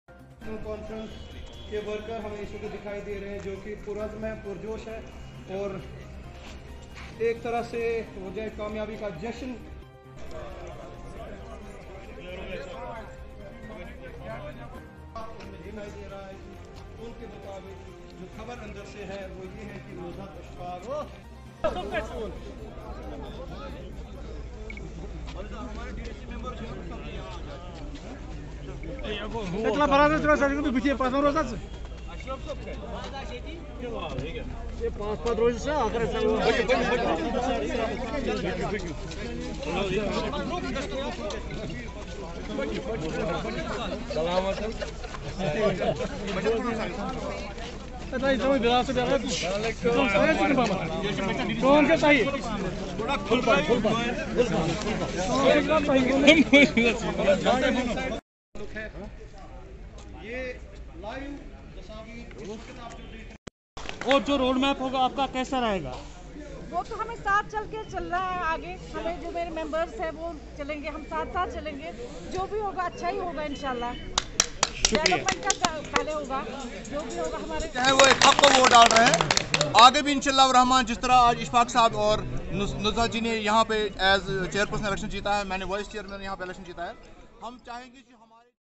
DDC Chairperson election: NC Candidate Nuzhat Ishfaq wins chairperson
DDC Chairperson election: NC Candidate Nuzhat Ishfaq wins chairperson, Bilal Ahmad PDP  as vice-chairperson

Ehsaan Yousf

Ganderbal: The PAGD on Wednesday won the chairman and vice-chairman posts in the DDC chairperson elections in central Kashmir’s Ganderbal district.

PAGD candidate Nuzhat Ishfaq polled 10 votes while her rival independent candidate Tasmeena Aadil secured 7 votes.
Reports said that the vice-chairman Bilal Ahmad got 10 votes.

They said that Nuzhat Ishfaq of Lar who had won DDC elections from Lar-1 block and will be the first chairperson of Ganderbal while Bilal Ahmad will be vice-chairperson.

It is pertinent to mention here that both PAGD candidates Have won the Chairperson and vice chairperson.